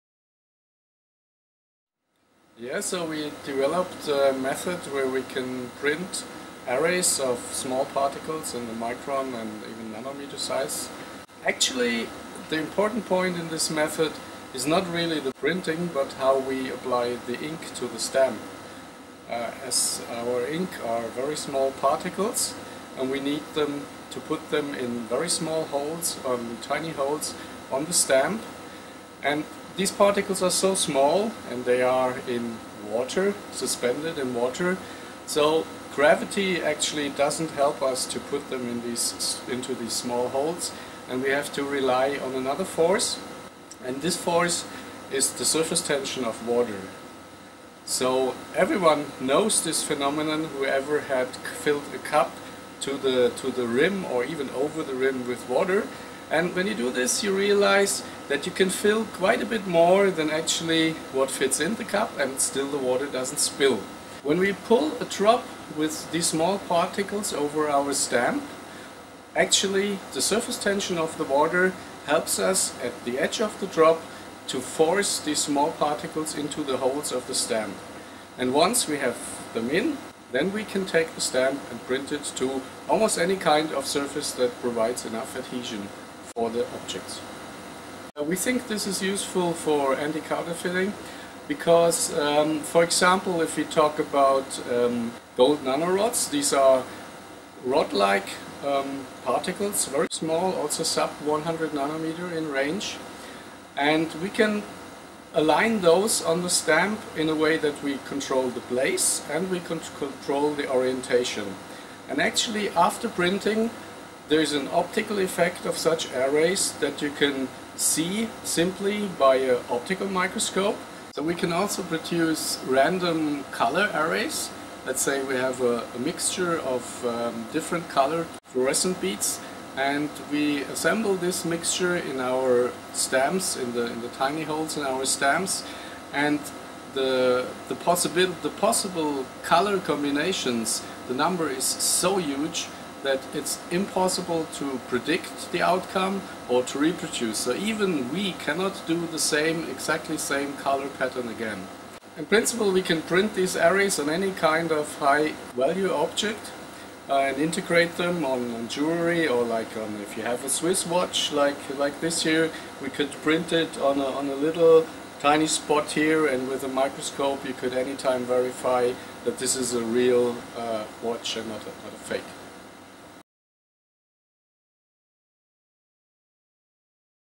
Nanorods Take Down Counterfeiters
IBM Research scientists in Zurich, have devised a new way to combat counterfeiting concept, by bringing the technology to an entirely different scale -- nano.

Earlier this year IBM scientists published a paper with the renowned university, ETH Zurich, demonstrating how they could precisely position gold nanorods, which measure 25 by 80 nanometers, on a surface using a simple printing process. As a point of comparison, the head of a pin is about 1 million nanometers wide.